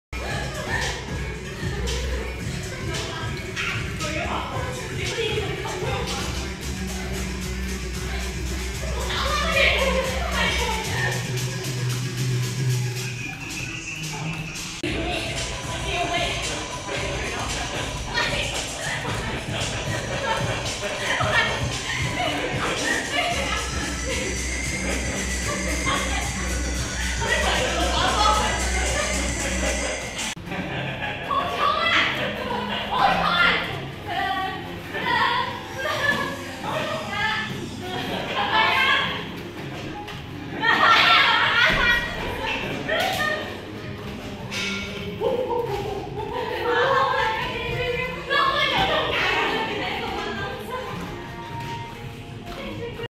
Cc&Toffee-體能堂之拋波波
Cc&Toffee為了保持良好的身形&健康的體魄，每星期都要進行訓練，希望展現最好的自己給觀眾。